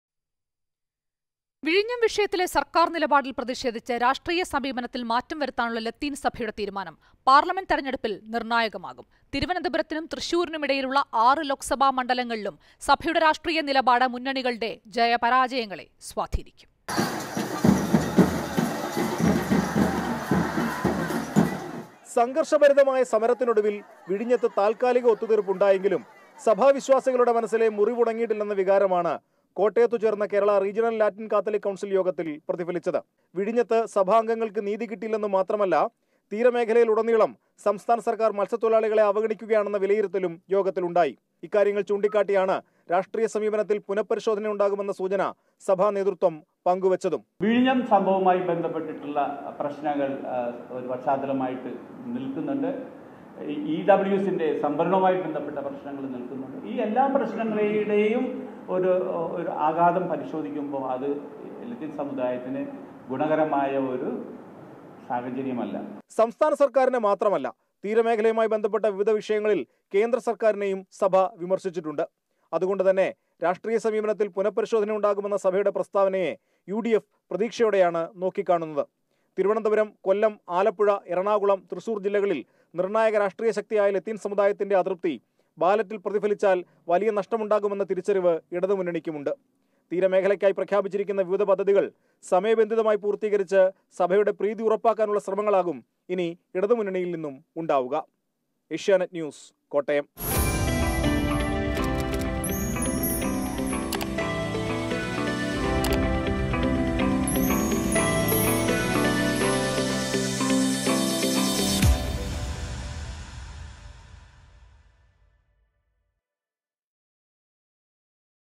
വിഴിഞ്ഞം സമരം;രാഷ്ട്രീയ നിലപാടില്‍ മാറ്റം വരുത്താന്‍ ഒരുങ്ങി ലത്തീന്‍ സഭ
വിഴിഞ്ഞം സമരത്തെ തുടര്‍ന്ന് രാഷ്ട്രീയ നിലപാടില്‍ മാറ്റം വരുത്താന്‍ ഒരുങ്ങി ലത്തീന്‍ സഭ. ഇത്  ലോക്‌സഭാ തെരഞ്ഞെടുപ്പില്‍ ഇടതുമുന്നണിക്ക് തലവേദനയാകും
vizinjam  protest | LDF | politics